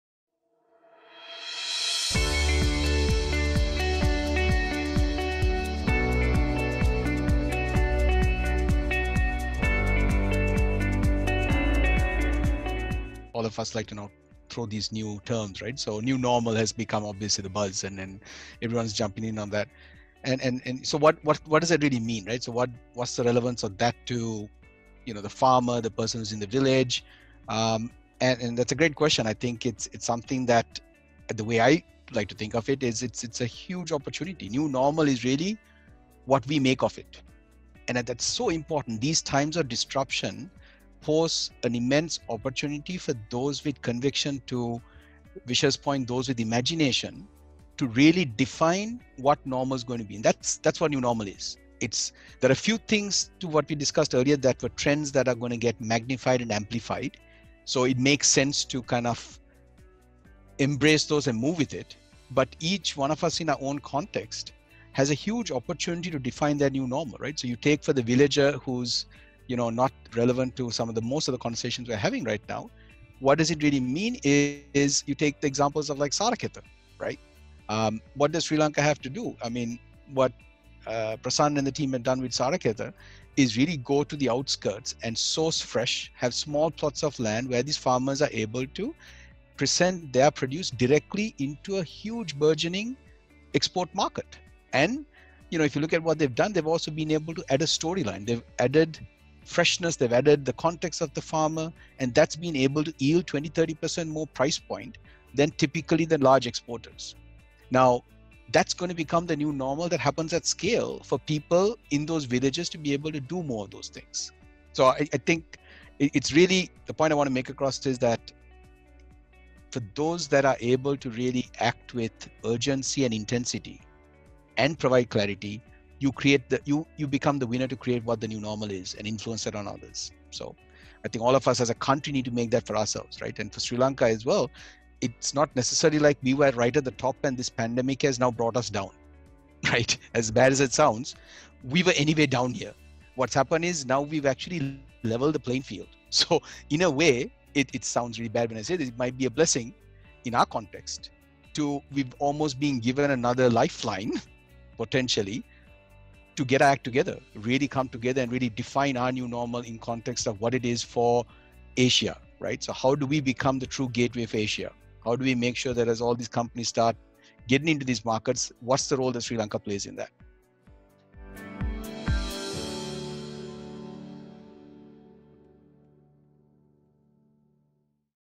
Another Lifeline to Bounce Back
Ruwindhu Peiris, Managing Director Stax Inc. shares his thoughts on how Sri Lanka can use the emerging 'New Normal' to recover and rebound in a post-Covid world.